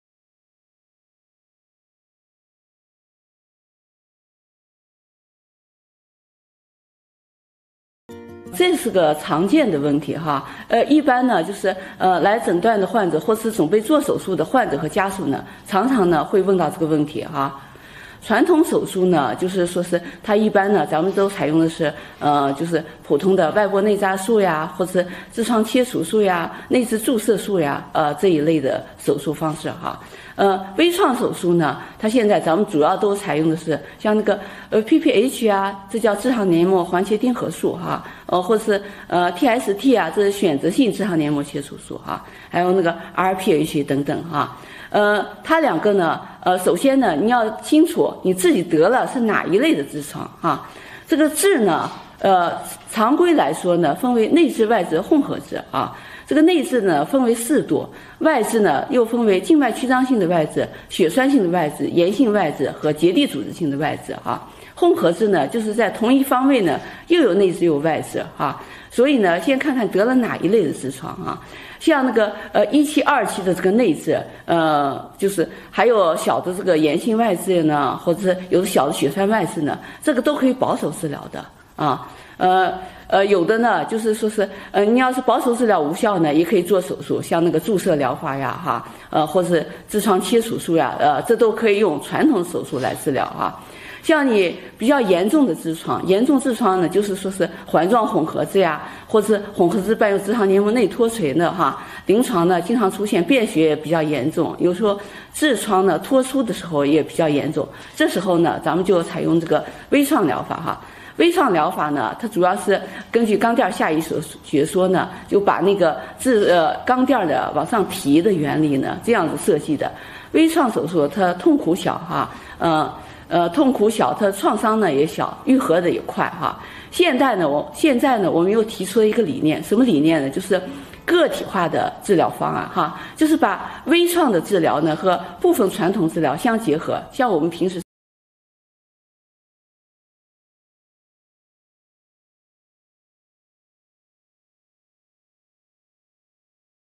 微创手术和传统手术治疗痔疮哪个比较好 韩丽君 山西中医学院附属医院
这是个常见的问题，一般来诊断的患者或是准备做手术的，患者和家属，常常会问到这个问题，传统手术就是说，一般都采用的是普通的外剥内扎术或是痔疮切除术，内痔注射术，这一类的手术方式，微创手术现在主要都采用的，像PPH这叫痔上黏膜环切术或是TST这是选择性，痔疮黏膜切除术，还有RPH等等它两个首先要清楚，自己得的是哪一类痔疮，痔常规来说分为内痔、外痔、混合痔，内痔分为Ⅳ度，外痔又分为静脉曲张性外痔，血栓性外痔、炎性外痔和结缔组织性外痔，混合痔就是在同一方位，又有内痔又有外痔。

Which is better between minimally invasive surgery and traditional surgery for hemorrhoids
This is a common question, Patients who are usually diagnosed or ready for surgery, Patients and family members, I often ask this question, Traditional surgery means, Generally, it is common external stripping and internal ligation or hemorrhoidectomy, Internal hemorrhoids injection, This kind of surgery, Minimally invasive surgery is now mainly used, Like PPH, which is called hemorrhoidectomy or TST, which is selective, Hemorrhoidectomy, And RPH and so on. First of all, they need to be clear, What kind of hemorrhoids do you have, Hemorrhoids can be divided into internal hemorrhoids, external hemorrhoids and mixed hemorrhoids, The internal hemorrhoids were divided into four degrees, External hemorrhoids can be divided into varicose external hemorrhoids, Thrombotic external hemorrhoids, inflammatory external hemorrhoids and connective tissue external hemorrhoids, Mixed hemorrhoids are in the same direction, There are internal hemorrhoids and external hemorrhoids.